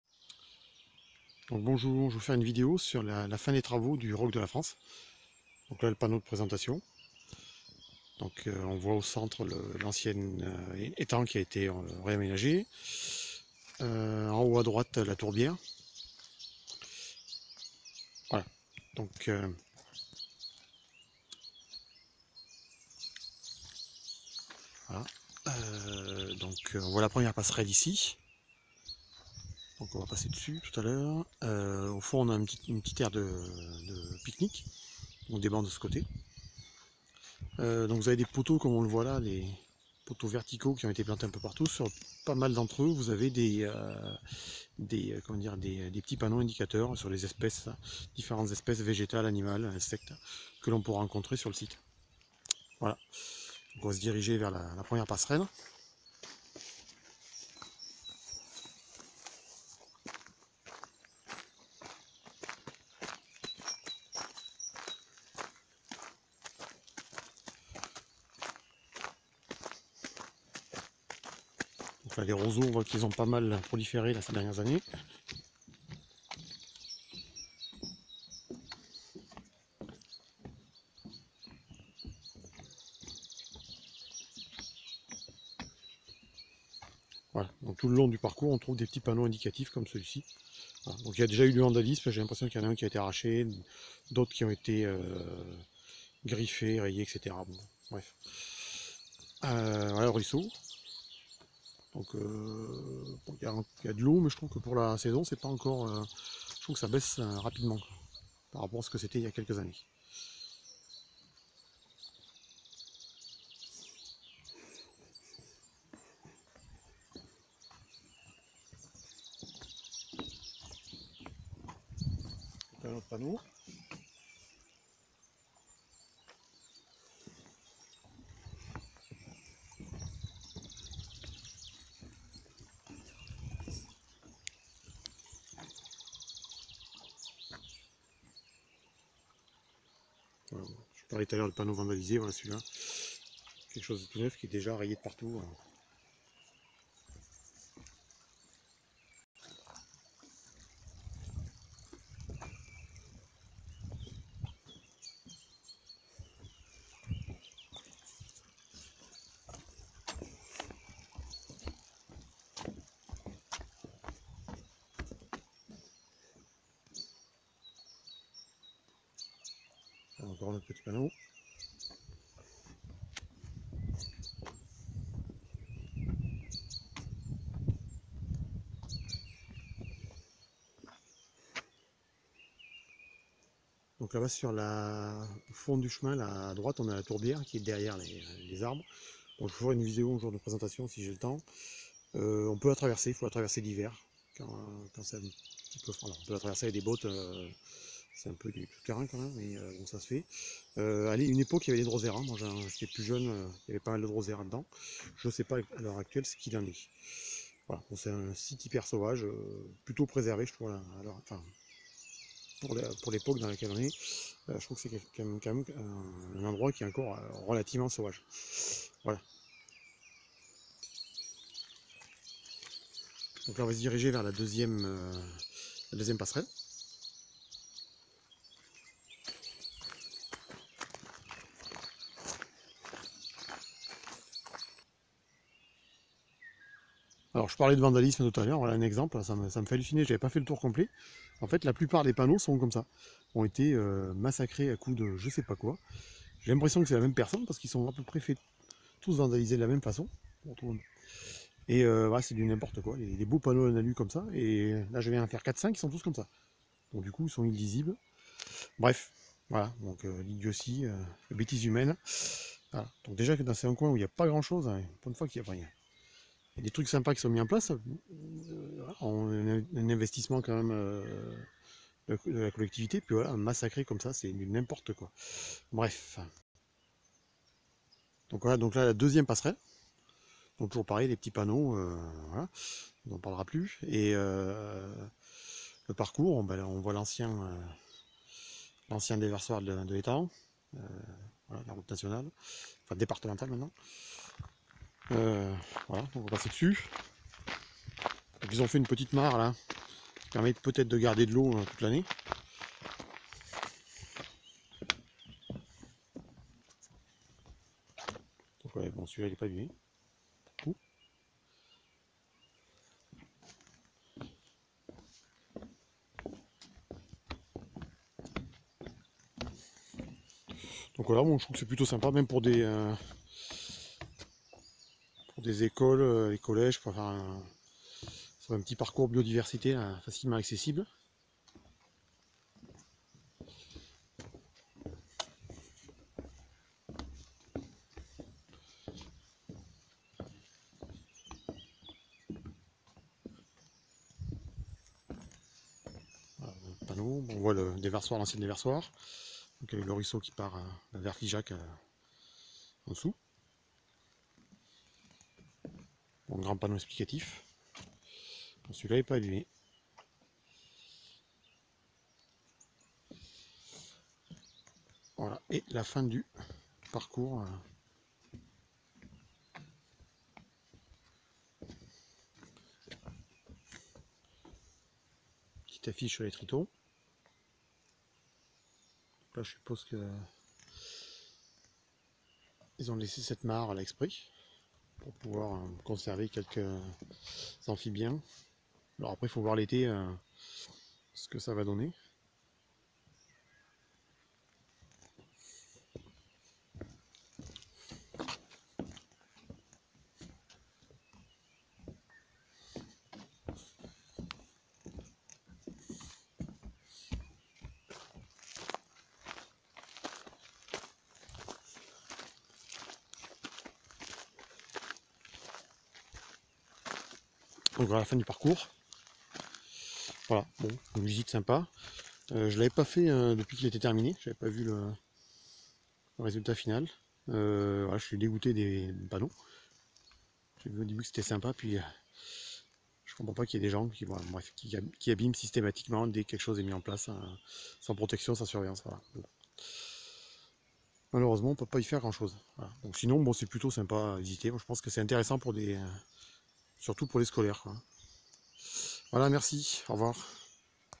Parcours biodiversité étang du Roc de la France à Latronquière
Présentation du parcours biodiversité installé à la place de l'ancien étang de Latronquière (Lot). Parcours pédagogique sur une zone humide.